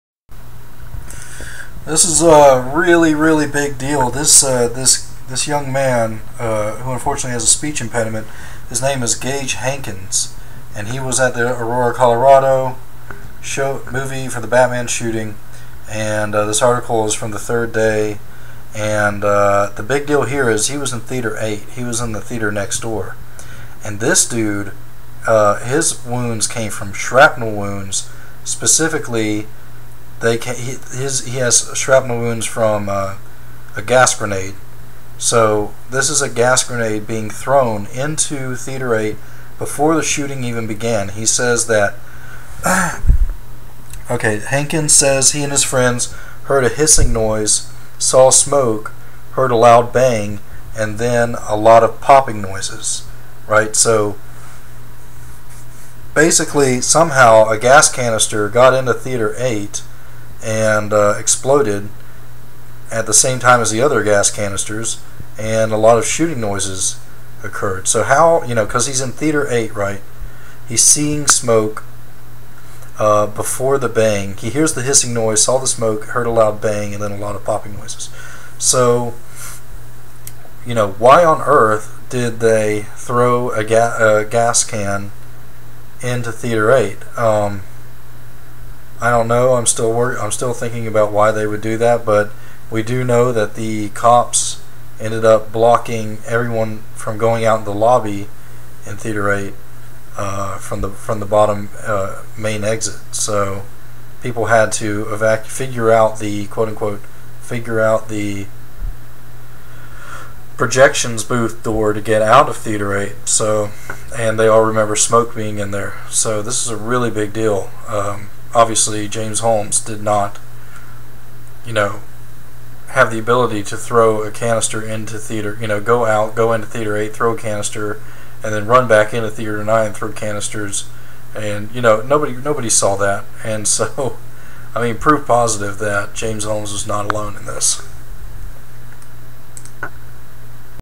Proof positive not a lone gunman in Aurora Batman Massacre (gas can went off in separate theater)
Final nail in the coffin with this one.  Gage Hankins WAS IN THEATER 8, and was injured by a gas grenade prior to the onset of gunshots.  Meaning a gas can went off in theater 8.

saw this interview, but it slipped by me that he was in theater 8.  Kudos to SheilaAliens and Christian Peterson (of HistoricalRecordsVLT), and to facebook groups who are sometimes referred to as "Holmies".

"Eighteen-year old Gage Hankins of Forest in northwest Ohio said in a televised news conference Monday the attack came about 20 minutes into the movie at a scene with gunshots.
Hankins says he and his friends heard a hissing noise, saw smoke, heard a loud bang and then a lot of popping noises.
Hankins says he thought it was special effects in the theater, then felt pressure on his arm and saw a hole.
He says a friend quickly got him out of the theater."

Said he had 3 pieces of shrapnel in him, and that they couldn't explain how it happened.  Yeah, probably because it is technically inexplicable under the official story.

Here is his speech therapist friend who was sitting next to him in theater 8, and she describes feeling the heat from the combustion of the grenade
UPDATE:  (SheilaAliens found this Tweet featured on KDVR's website regarding theater 8's smoke bombs)

"Our theater: bullets came through the walls and hit some people in our theater, and smoke bombs were thrown into our theater." - account: "Rachel Fedeli"